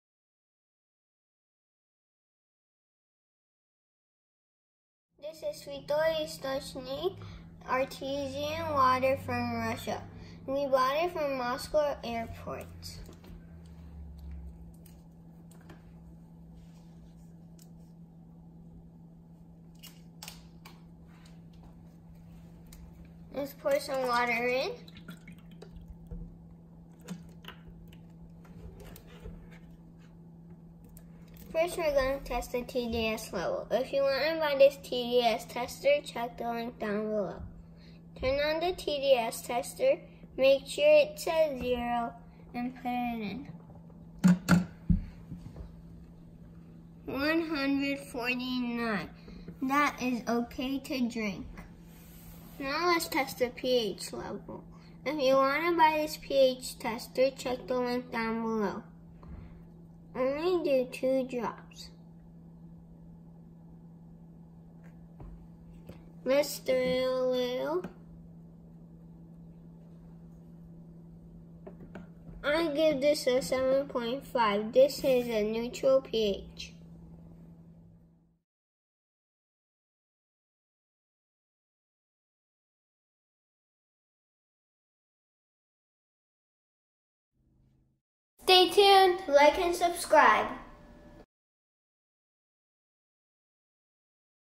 Святой Источник - Svyatoy Istochnik Artesian Water test - pH and TDS
Artesian water from Russia.

Results:
pH: 7.5
TDS: 149